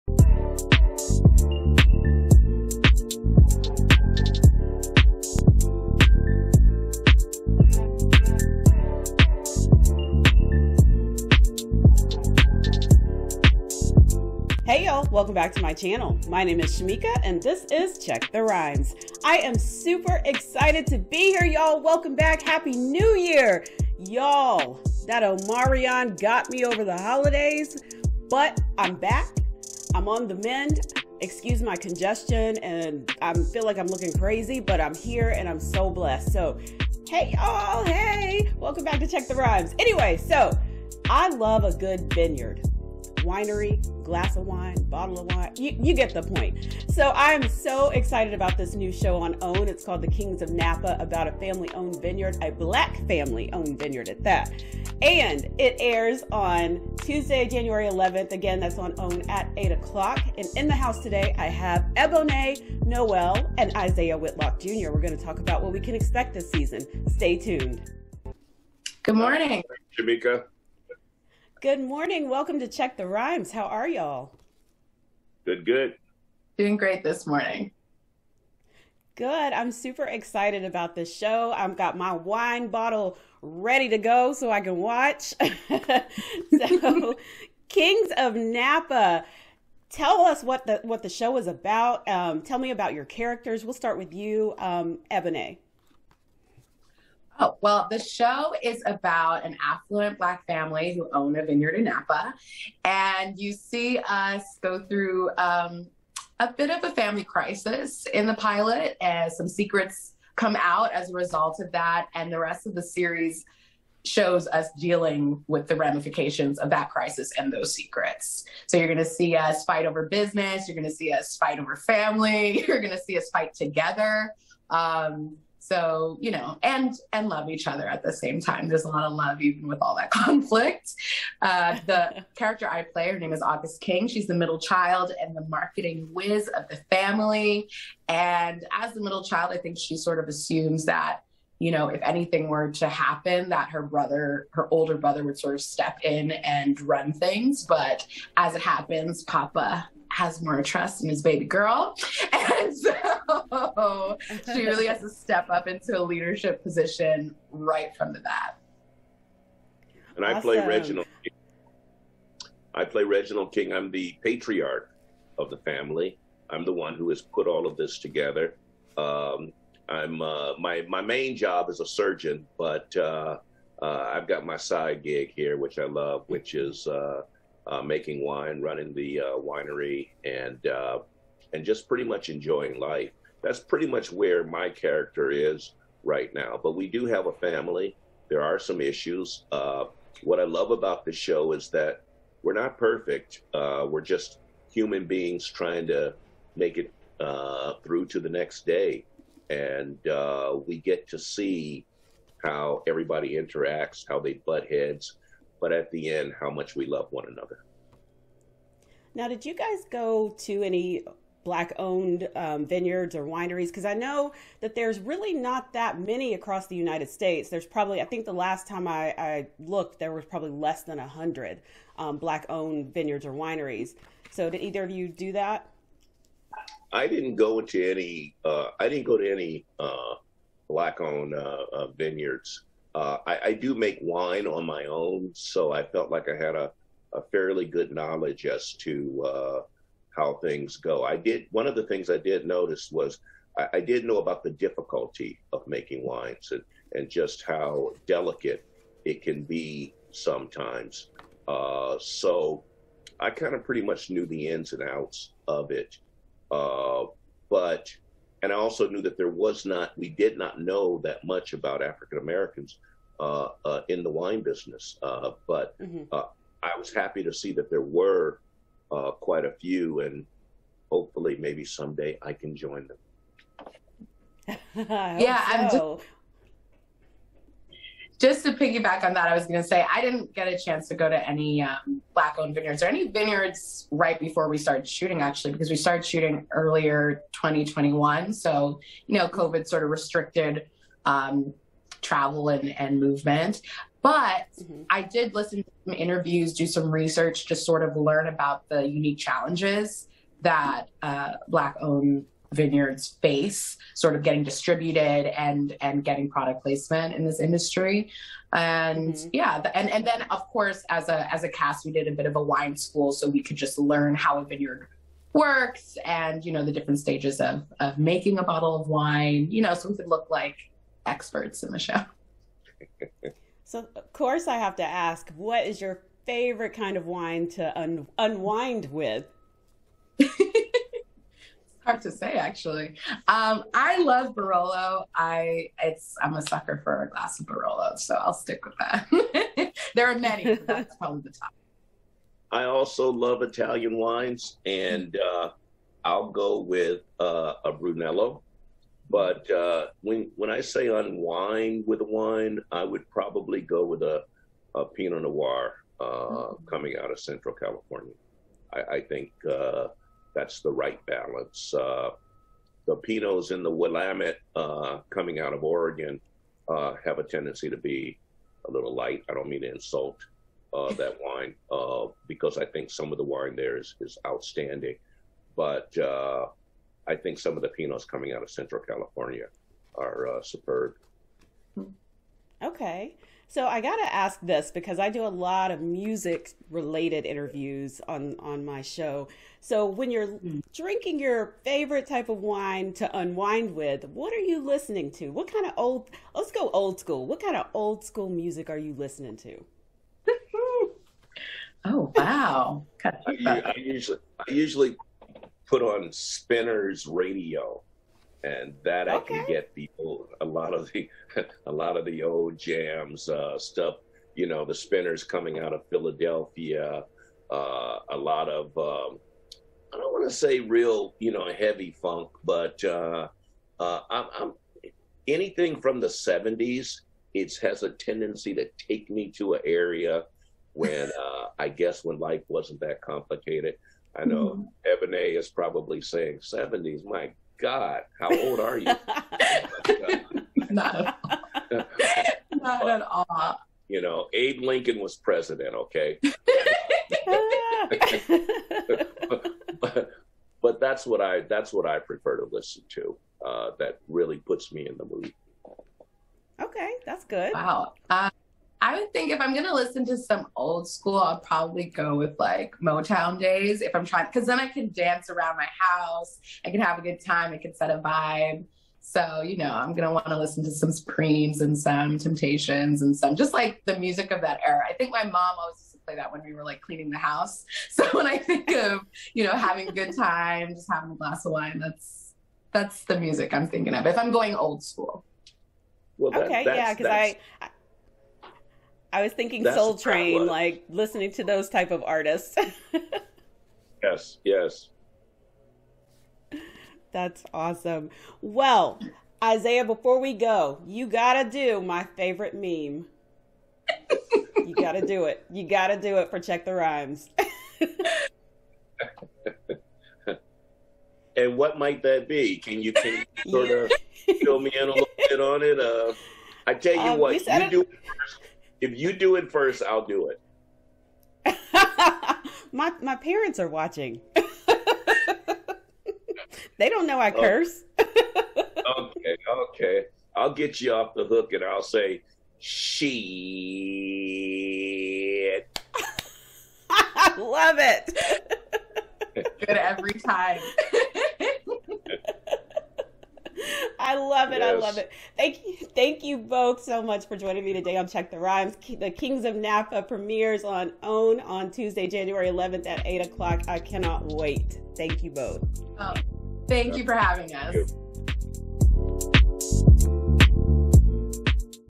Ebonée Noel and Isiah Whitlock Jr. talk wine on the new OWN show "The Kings of Napa"
The Kings of Napa is centered on a gorgeous and picturesque Napa Valley, California, vineyard owned by the Kings, an aspirational African American family whose wealth and status land them on the pages of design magazines and society pages. The wine business has brought the family success and acclaim, but following the patriarch’s sudden exit from the company, his three children must grapple for the reins to the kingdom — to their own power, wealth, and legacy. Ebonée Noel and Isiah Whitlock, Jr. stopped by Check the Rhymes to talk about the new OWN series that premieres on 1/11, and their favorite wines!

Catch me on Zoneify on the Expression Channel just type in Check the Rhymes